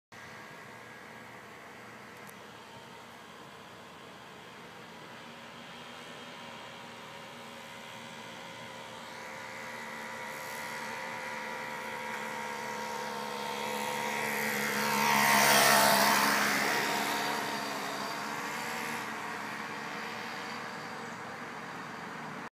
Weed wacker bike
This is a video of me riding my weed wacker bike in the middle of the road.